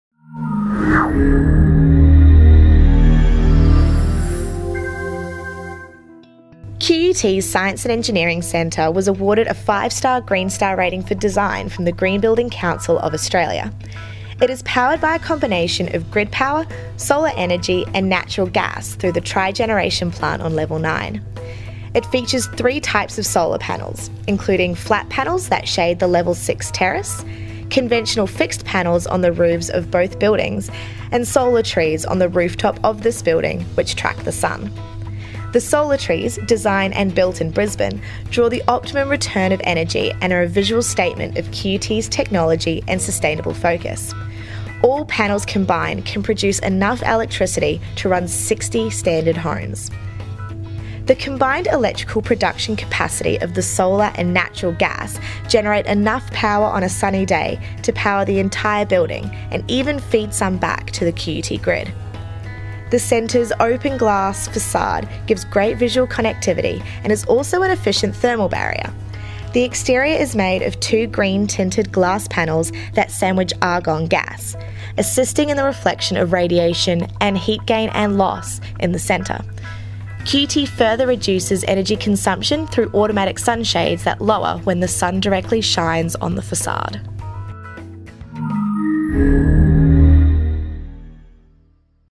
QUT P Block Digital Tour - Renewable Energy Features
A digital tour of P Blocks renewable energy features, including solar gardens, solar tree's, and a variety of other green technologies.